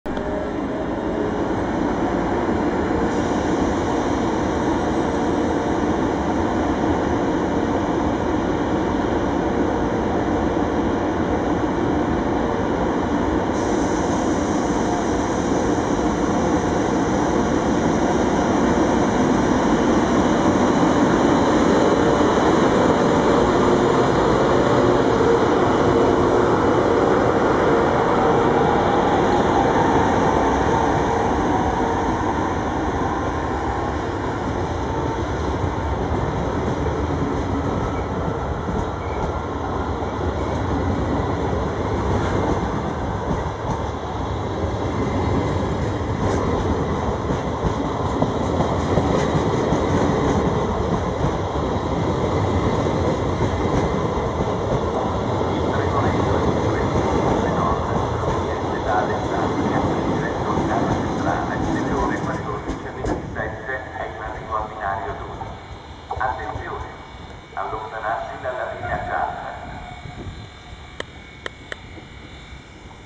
Tigre in soccorso (E633.092 + 7 MDVC + E464.322 Trenord)
L'unità E633.092 soccorre un convoglio MDVC Trenord.

Milano Lambrate, 14/6/2013